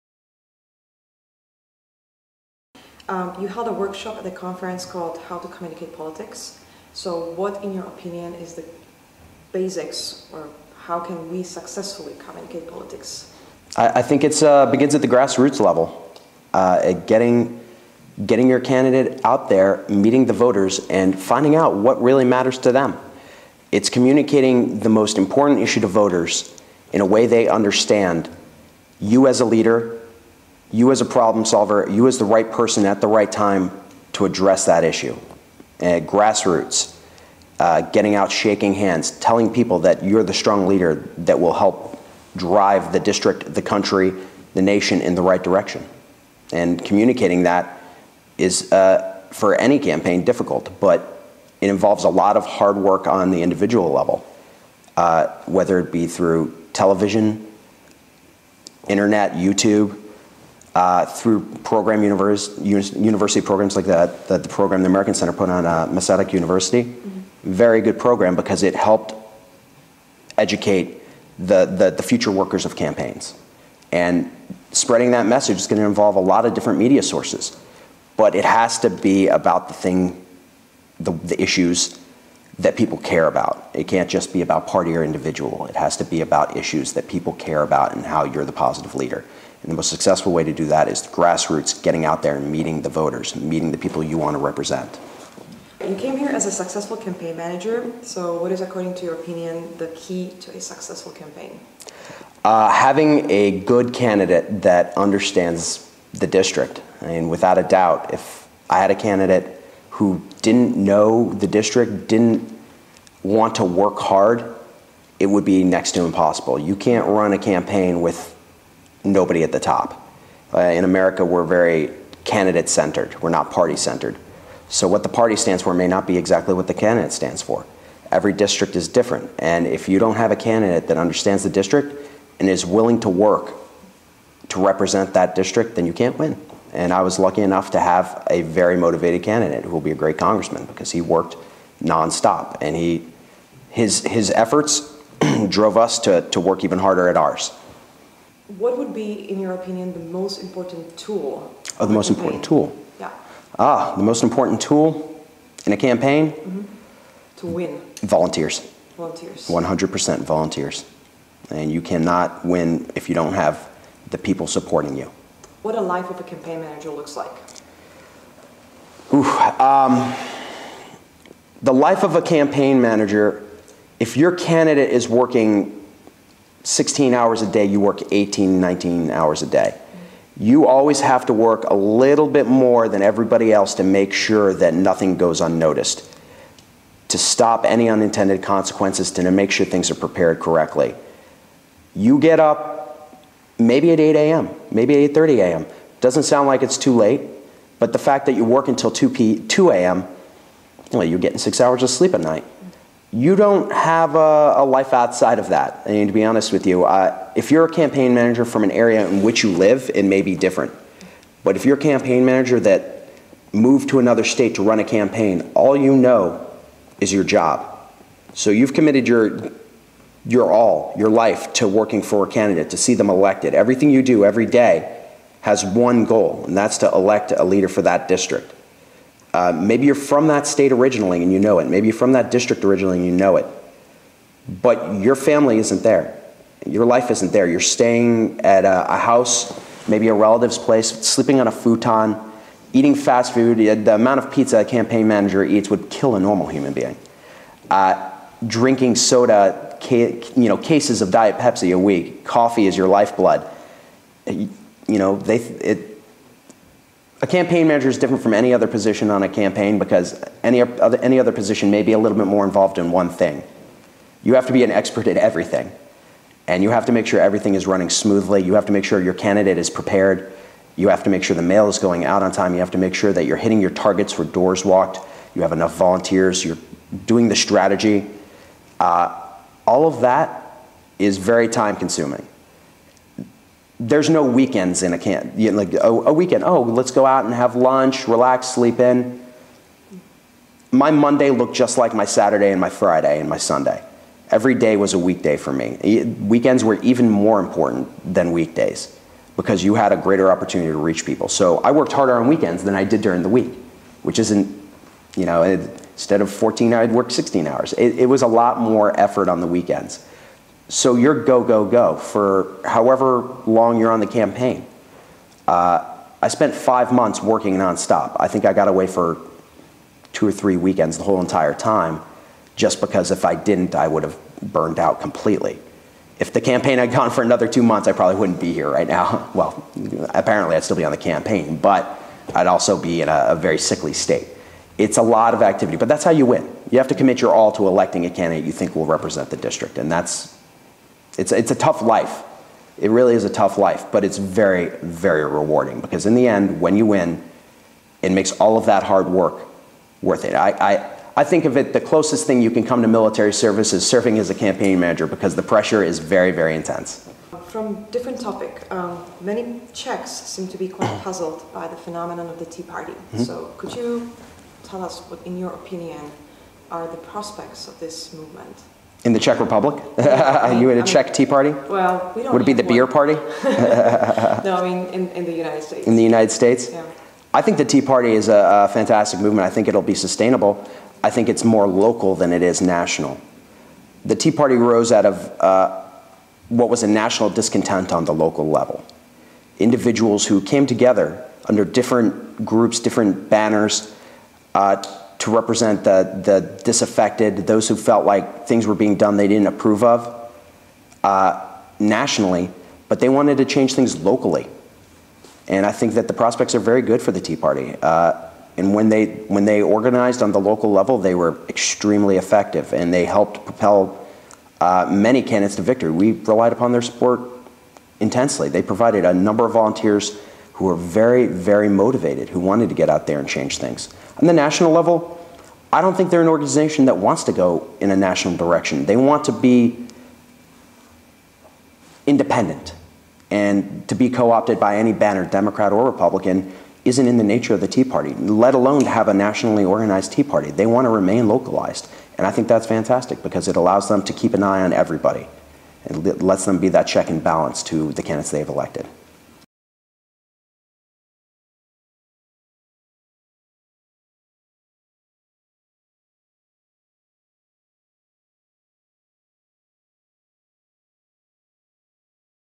Political Campaigns in the US
Interview with the US campaign manager Aaron E. Ringel who ran succesfull campaign of Republican Michael Grimm in midterm elections 2010.

Aaron E. Ringel participated at the conference "How to Communicate Politics" at the Faculty of Social Studies of the Masaryk University in Brno on November 22nd, 2010 and held a workshop on campaign organization and strategy, with a particular focus on getting out the vote (GOTV) at the American Center November 24th, 2010.